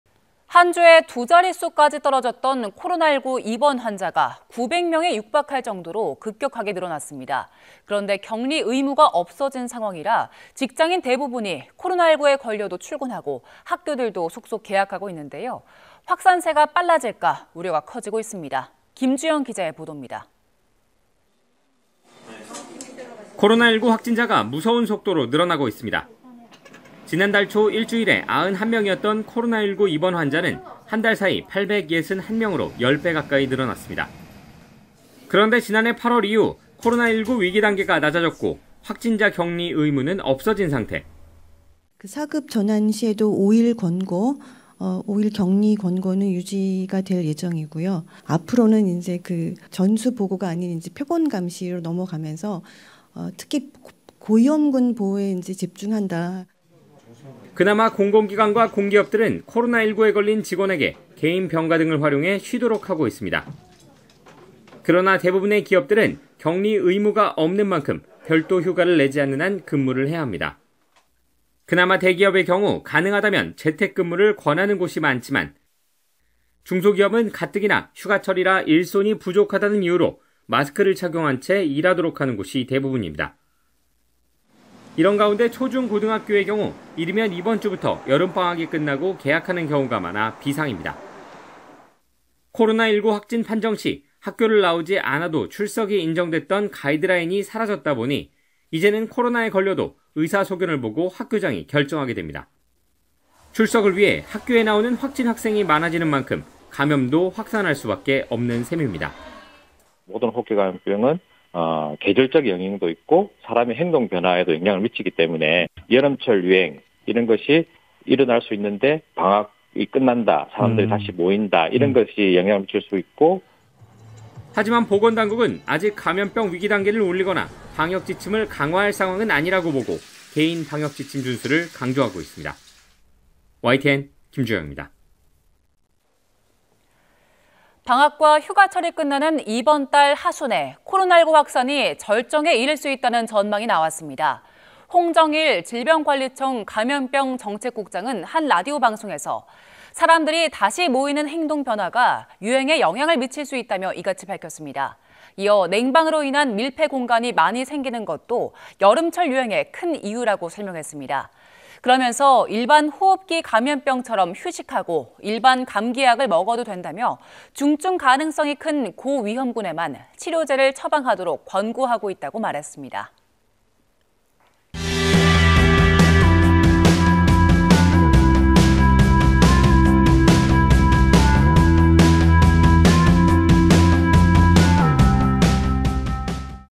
"휴가 내기 아까워 출근"…개학까지 겹쳐 확산 비상 [세상만사]
한 주에 두 자릿수까지 떨어졌던
코로나19 입원 환자가
9백 명에 육박할 정도로
급격하게 늘어났습니다.

그런데 격리의무가 없어진 상황이라
직장인 대부분이
코로나19에 걸려도 출근하고,
학교들도 속속 개학하고 있는데요.

확산세가 빨라질까
우려가 커지고 있습니다.